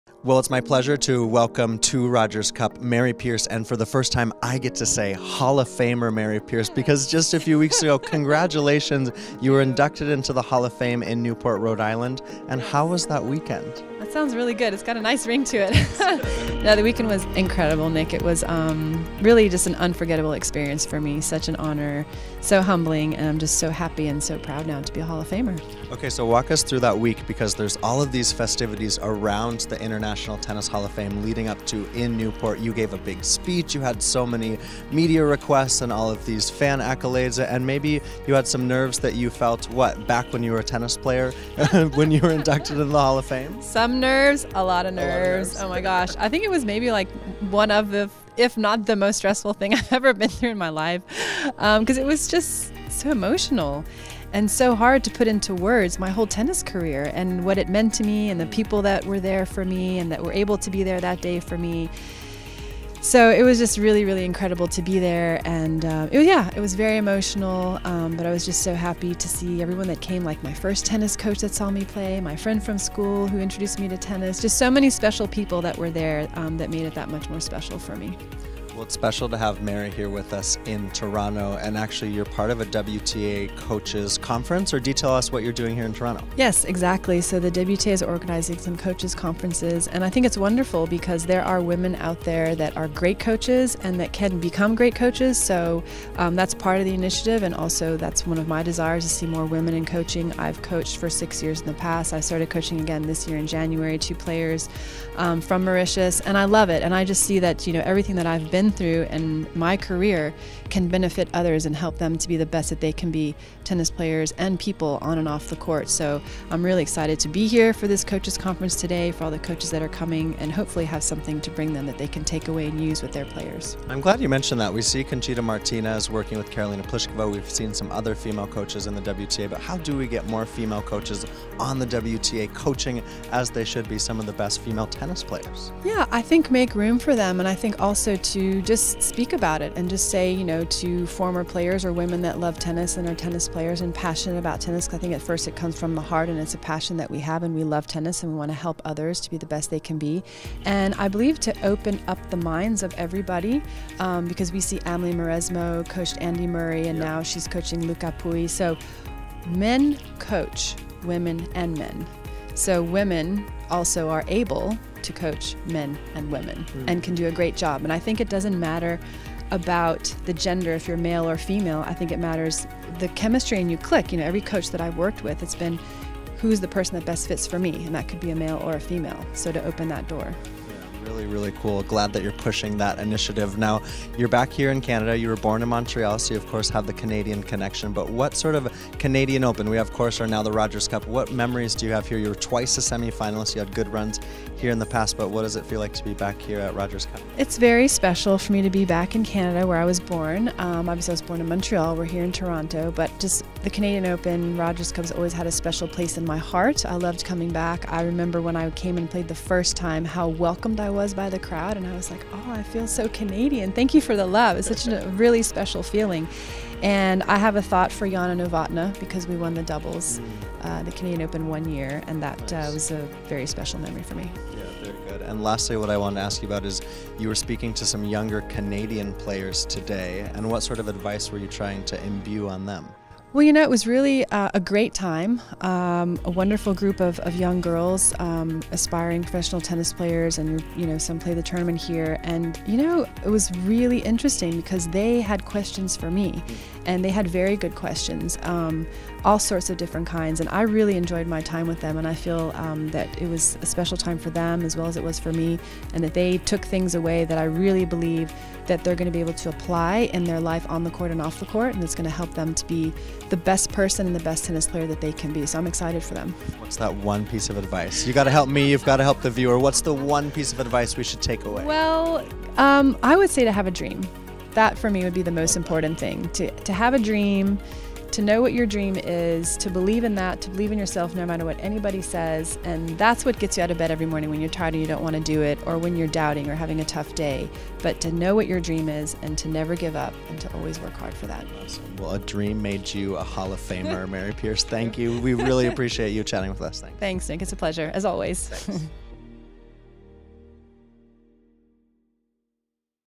Interview: Catching up with hall-of-famer Mary Pierce at Rogers Cup
Montreal-born Mary Pierce is a two-time Grand Slam champ and a doubles champ in Toronto 20 (!!) years ago.

Inducted into the International Tennis Hall of Fame last month, we had a chance to catch up with her back on familiar soil.